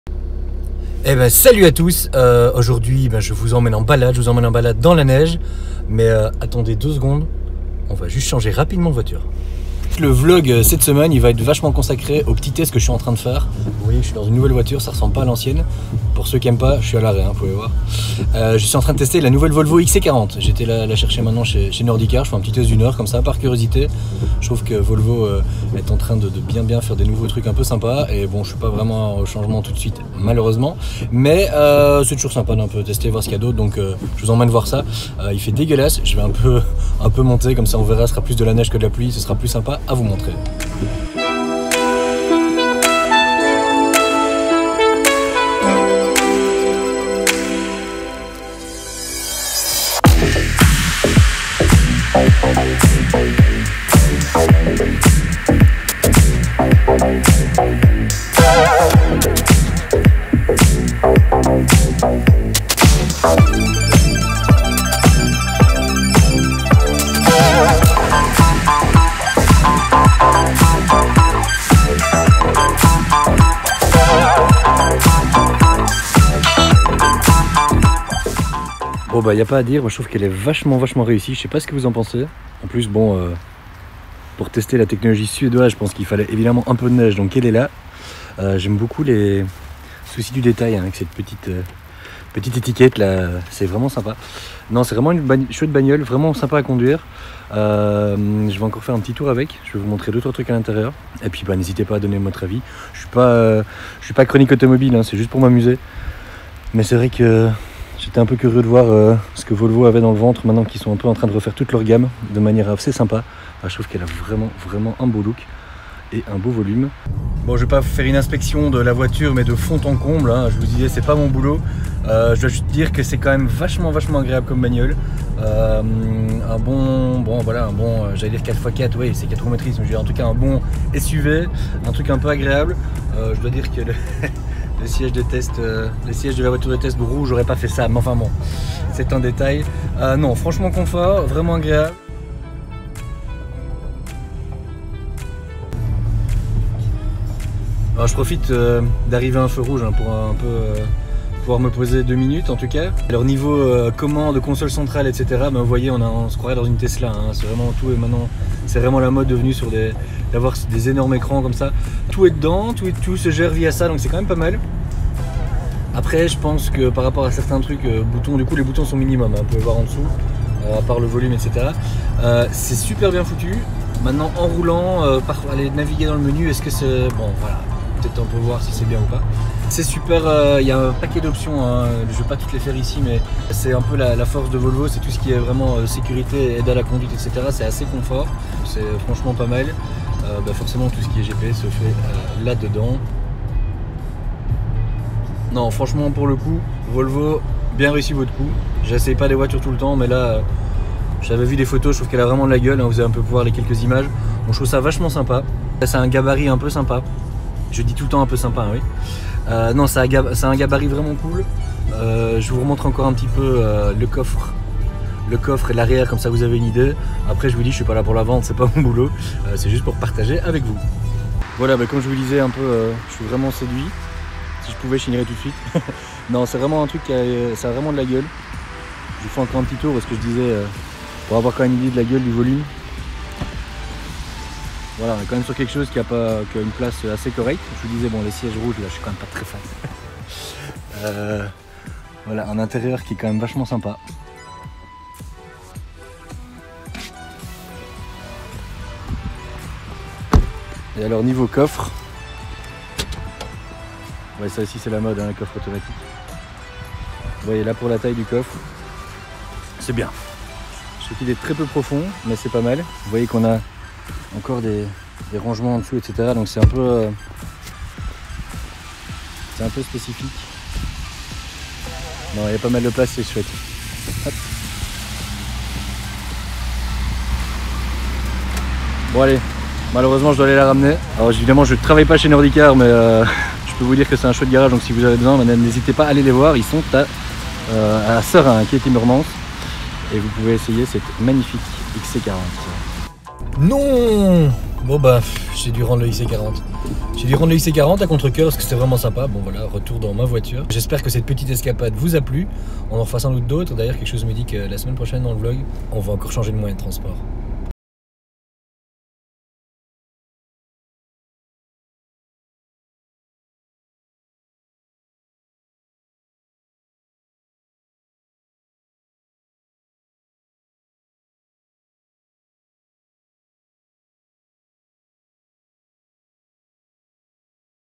TEST : MON ESSAI DE LA VOLVO XC40 🚙 — VLOG #14
Curieux d'en voir + ? Foncez au garage Nordicar à Liège
Rode Videomicro
DJI Osmo Mobile